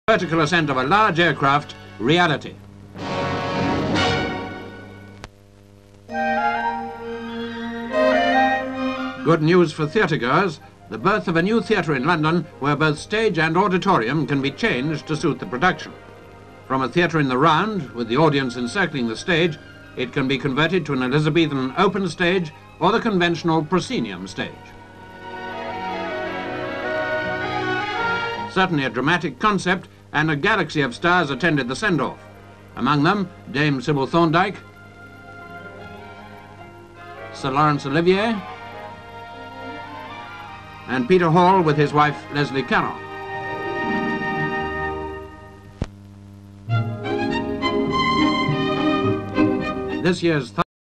"CONVERTIBLE" THEATRE
The birth of a new theatre in London, where both stage and auditorium can be changed to suit the production. Certainly a dramatic concept and a galaxy of stars attended the send off. Among them, Dame Sybil Thorndike, Sir Laurence Olivier, Peter Hall and his wife Leslie Caron.